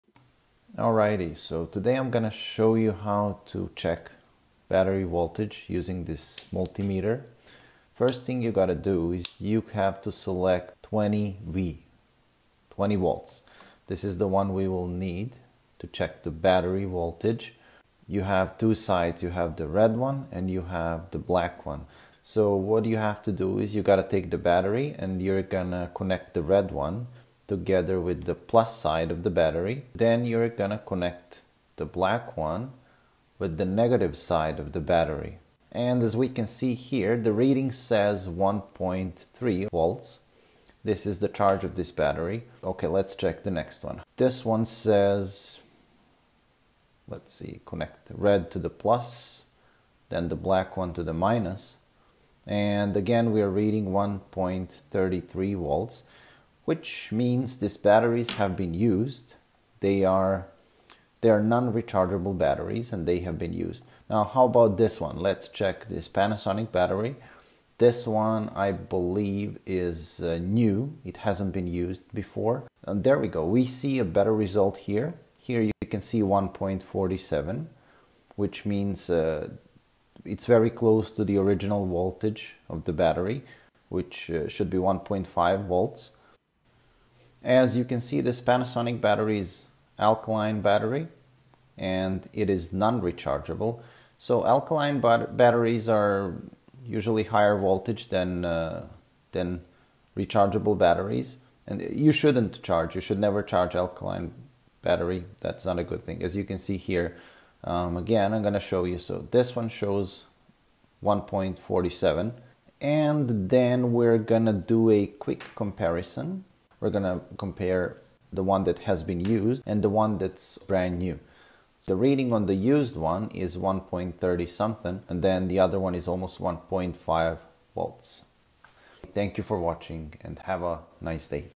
How to Check AAA or AA Batteries Using Multimeter
How to check your AAA or AA batteries Using Multimeter.